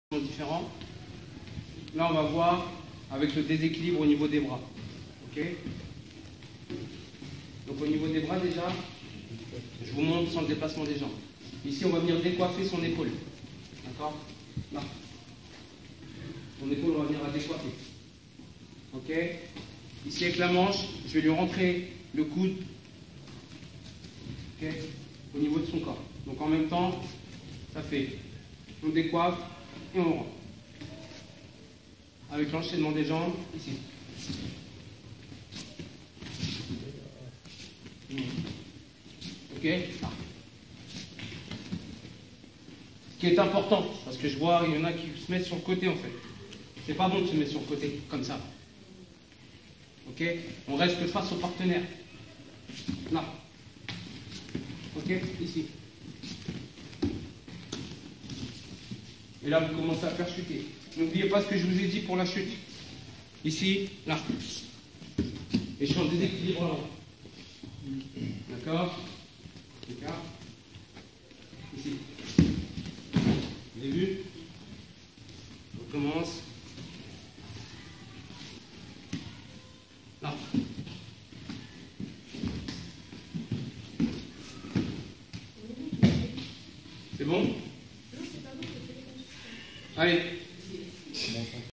O uchi gari : déséquilibre au niveau des bras
O uchi gari : démonstration du déséquilibre au niveau des bras dans la forme fondamentale de la technique